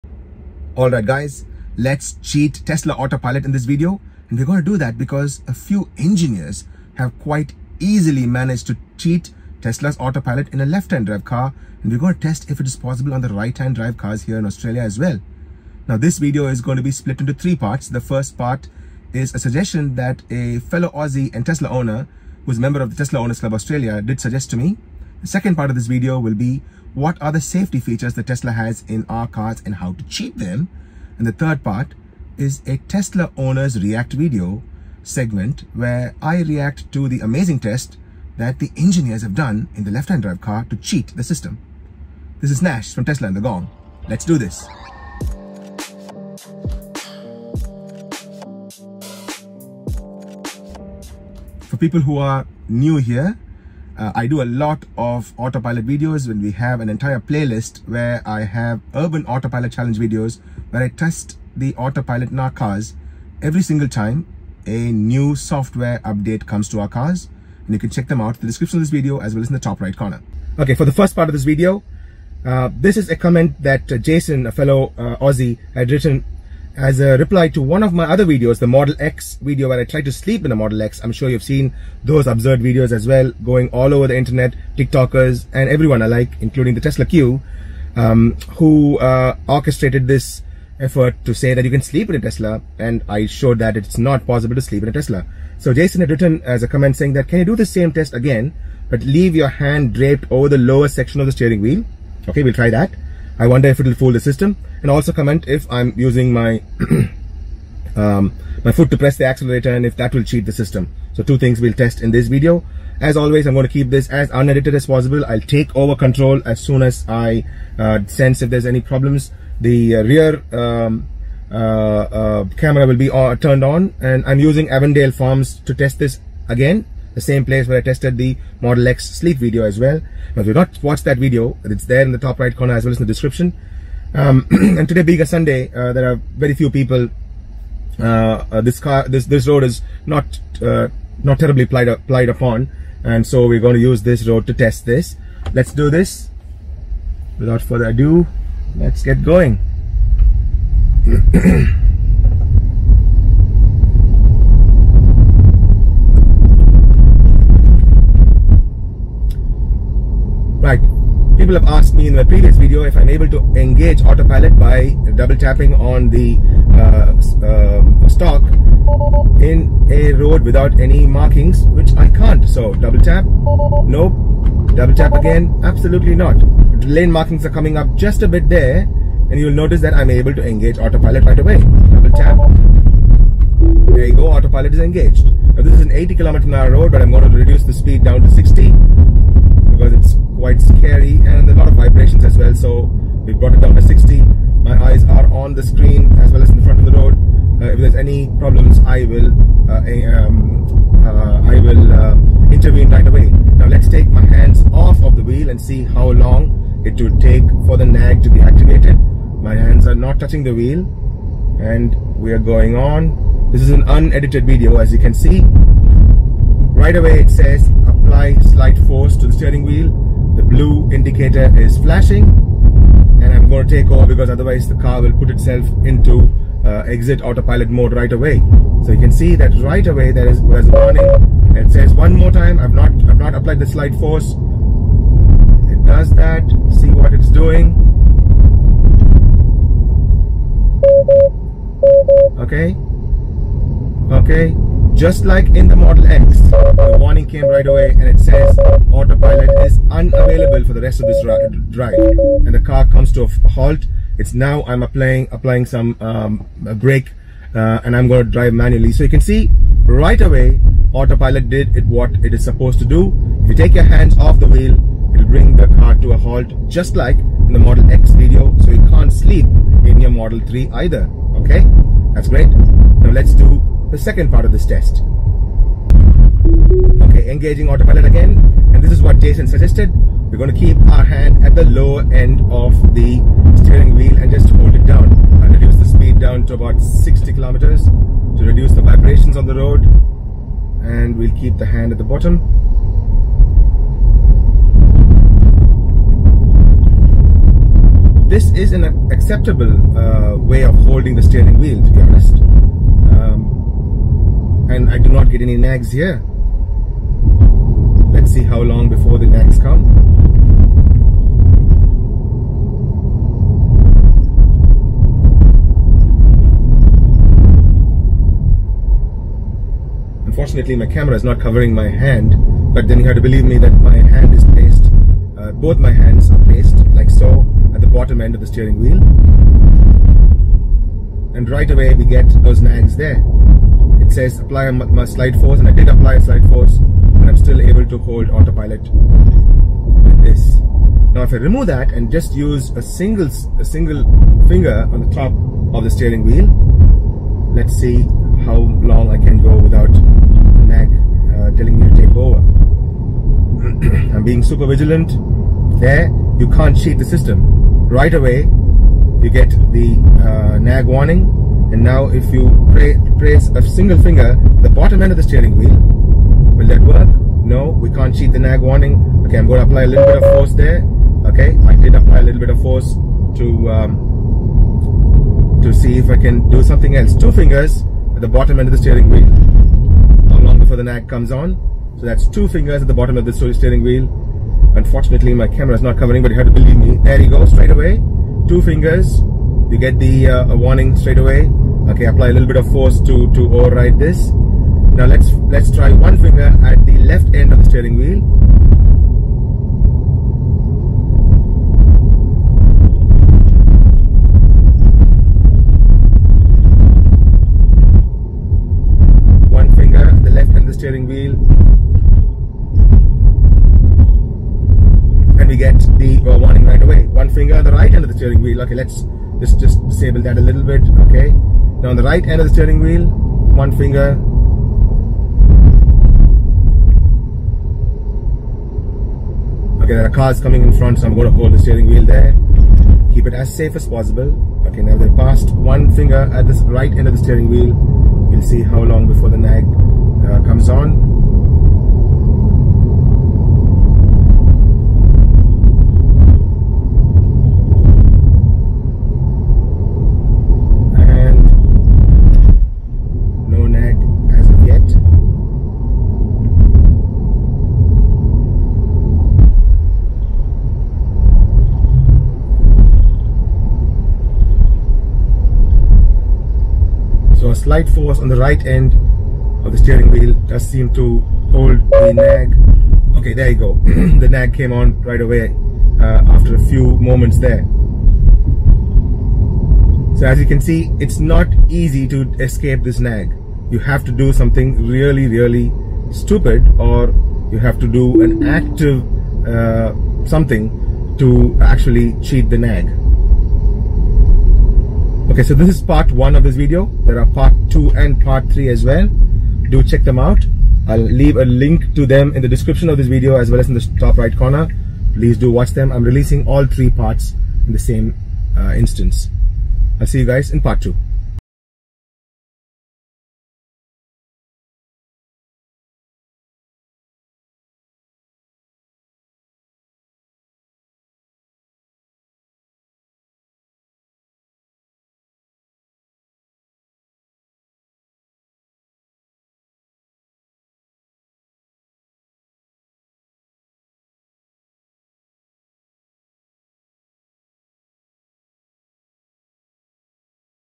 How to cheat Tesla Autopilot - is it even possible ? - Part 1 of 3
Is it possible to cheat @TeslaMotors  Autopilot. In this Part 1 video we see how to cheat Tesla Autopilot - is it possible Part 1

Thank to @consumerreports  for their inspiration !

CHAPTERS:
Introduction : 0:00
How autopilot actually works : 2:25
User Suggested Holding the lower third of the steering - 4:38
Single finger cheat - 5:44
Outro - 8:14

HIRE MY TESLA MODEL 3 SR+ Ex-Canberra

📷 Instagram : Tesla in the Gong